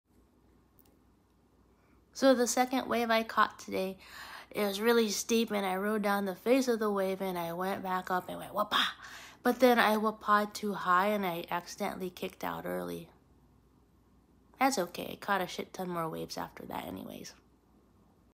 Shark Stories - Accidental Early Kickout
Surfed Doheny today. Early kickout on my second wave.